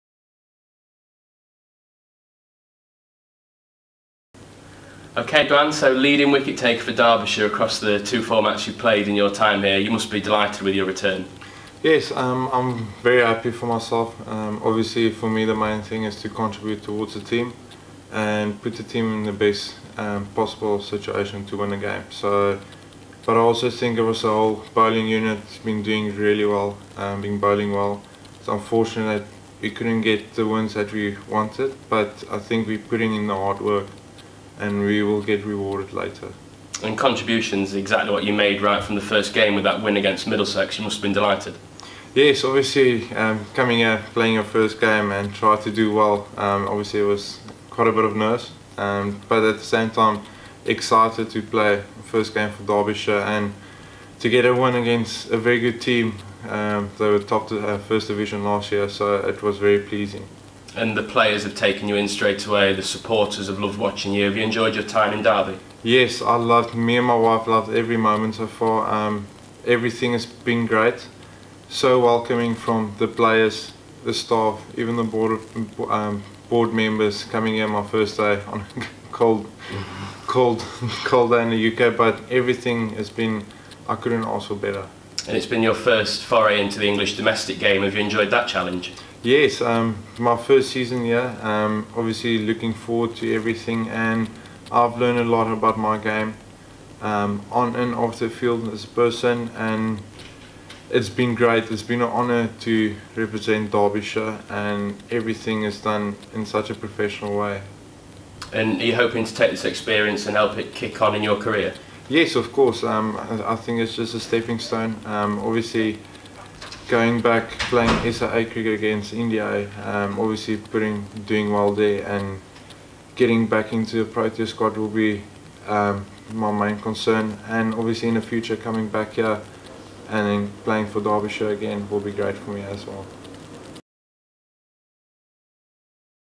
INTERVIEW: Duanne Olivier chats about his spell at Derbyshire
DerbyshireTV spoke to South Africa international Duanne Olivier after his spell at Derbyshire for the first half of 2018 season.